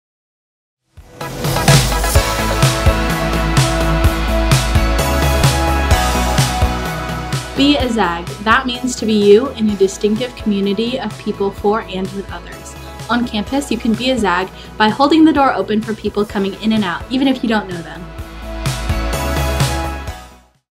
HowToZag: "Be a Zag"
Being a Zag is something different for
everyone. There is no one-size-fits-all
type of person at Gonzaga, as you are
invited to be YOU at GU.